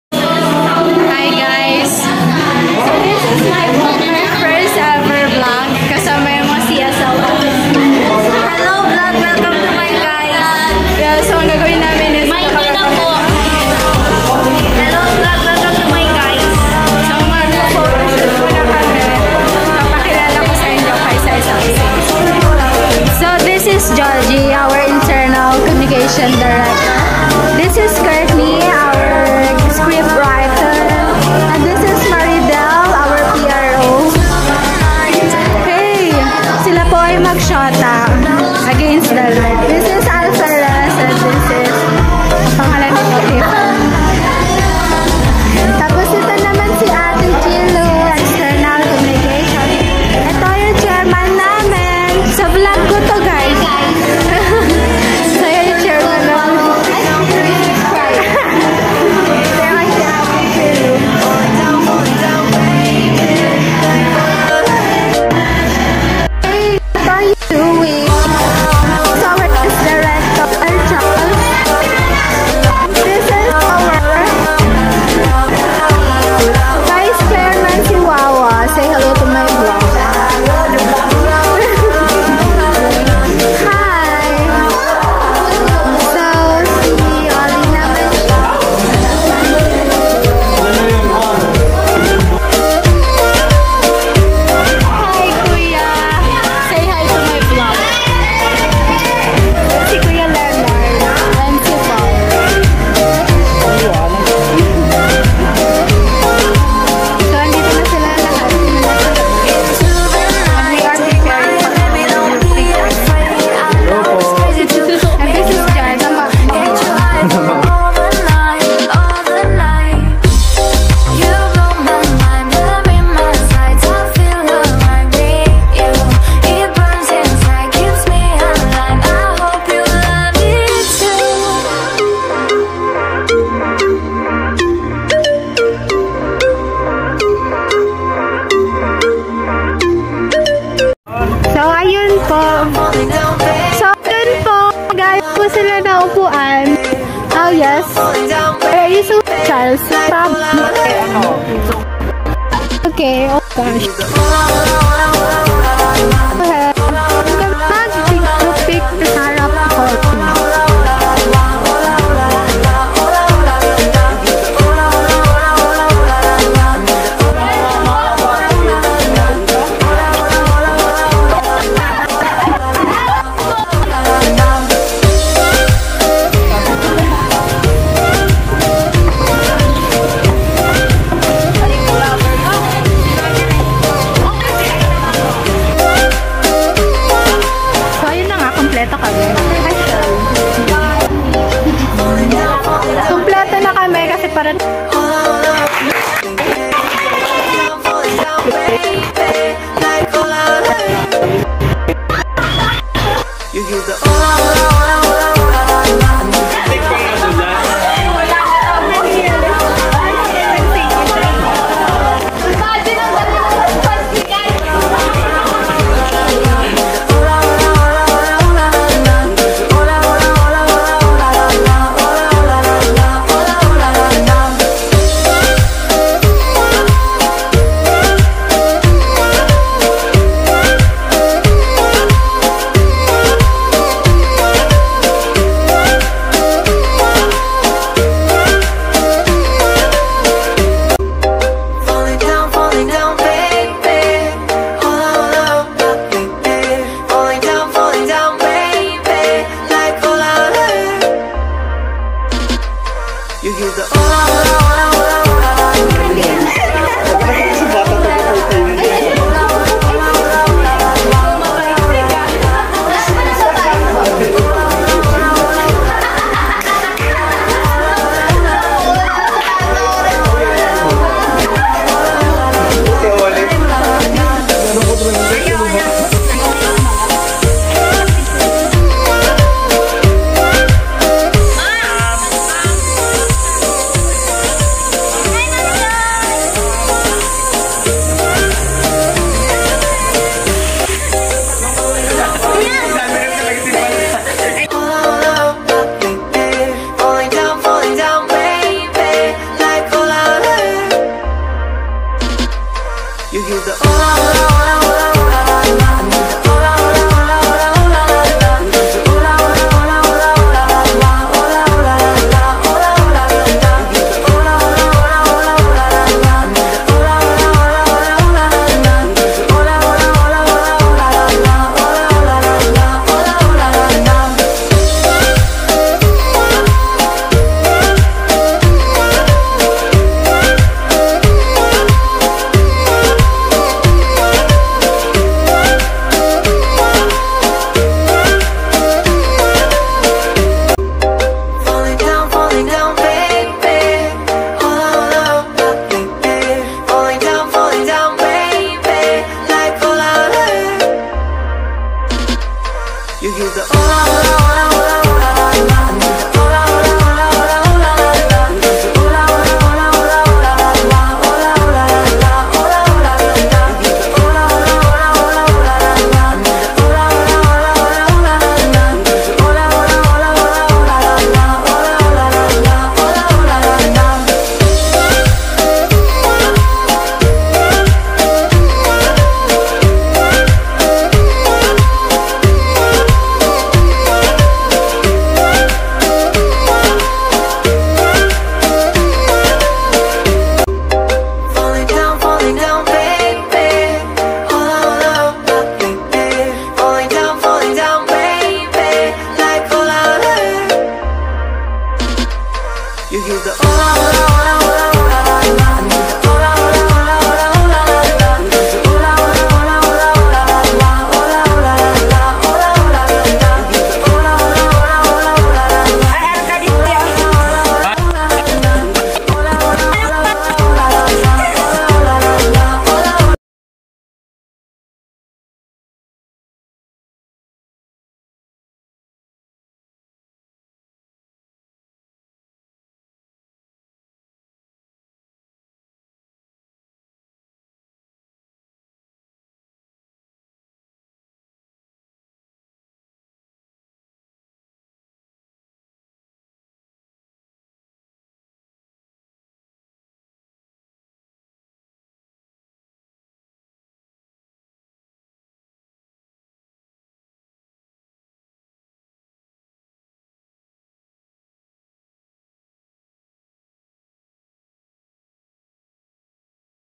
CSL 2022-2023 GROUP PICTURE
It is my first time to upload a video containing my fellow CSL officers. Please do watch and know each of them☺️😁

photo credits: Kevin Hernandez